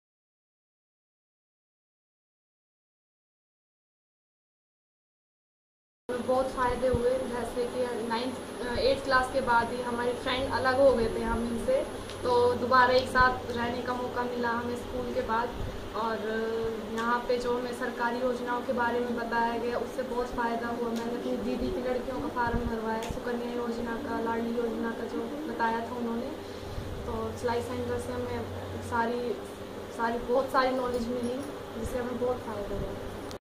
Lalita's quest for a better life
Life Skills Education Centers organized by Sehgal Foundation have transformed the lives of many adolescents girls in rural villages of Mewat, Haryana.